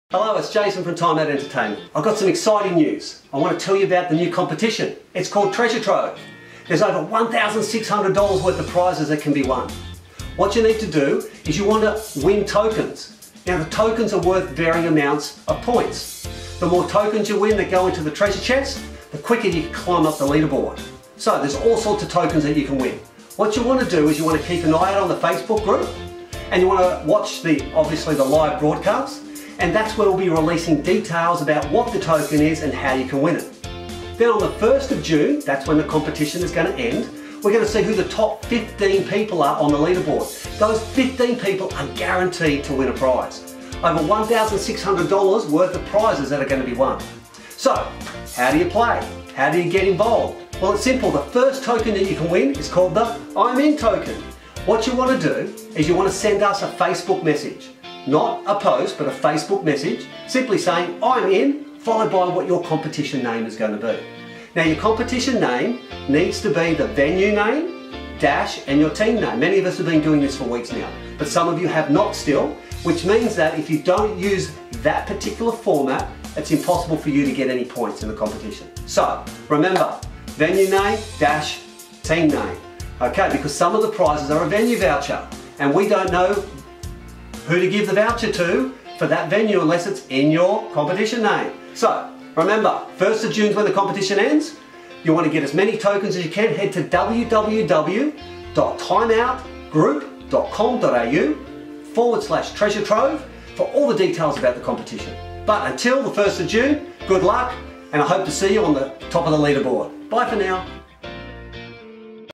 Introduction to the Treasure Trove Competition
This video will explain what the Treasure Trove competition is and how to play.  Competition ends 1st June 2020.

**NOTE** This competition is only for residents of Australia